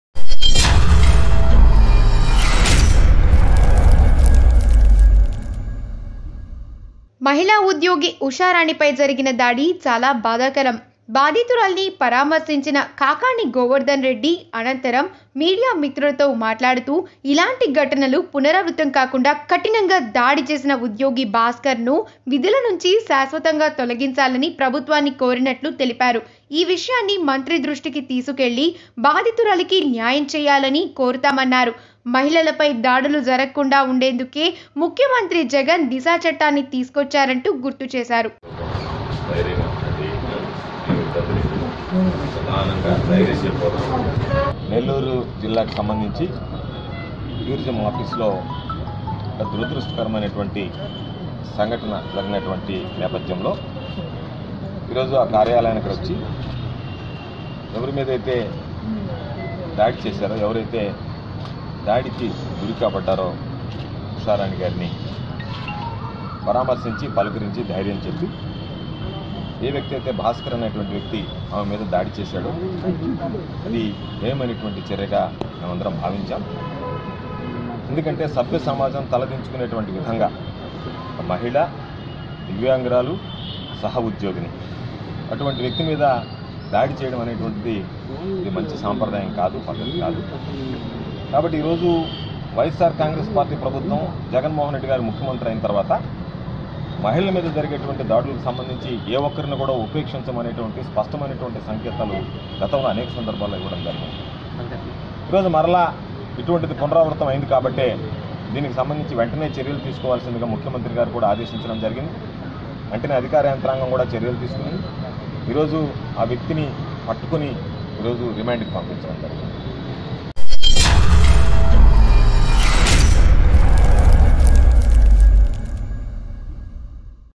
మహిళా ఉద్యోగి ఉషారాణిపై జరిగిన దాడి చాలా బాధాకరం|సర్వేపల్లి ఎమ్మెల్యే కాకాణి | BKRNEWS
బాధితురాలిని పరామర్శించిన కాకాణి గోవర్ధన్ రెడ్డి అనంతరం ఆయన మీడియాతో మాట్లాడారు. ఇలాంటి ఘటనలు పునరావృతం కాకుండా కఠినంగా దాడి చేసిన ఉద్యోగి భాస్కర్ ను విధుల నుంచి శాశ్వతంగా తొలగించాలని ప్రభుత్వాన్ని కోరినట్లు తెలిపారు. ఈ విషయాన్ని మంత్రి దృష్టికి తీసుకెళ్ళి బాధితురాలికి న్యాయం చేయాలని కోరుతామన్నారు. మహిళలపై దాడులు జరగకుండా వుండేందుకే ముఖ్యమంత్రి జగన్ దిశ చట్టాన్ని తీసుకొచ్చారని గుర్తు చేశారు.మహిళా ఉద్యోగి ఉషారాణిపై జరిగిన దాడి చాలా బాధాకరం,సర్వేపల్లి ఎమ్మెల్యే కాకాణి గోవర్ధన్ రెడ్డిమహిళా ఉద్యోగి ఉషారాణిపై జరిగిన దాడి చాలా బాధాకరం,సర్వేపల్లి ఎమ్మెల్యే కాకాణి గోవర్ధన్ రెడ్డి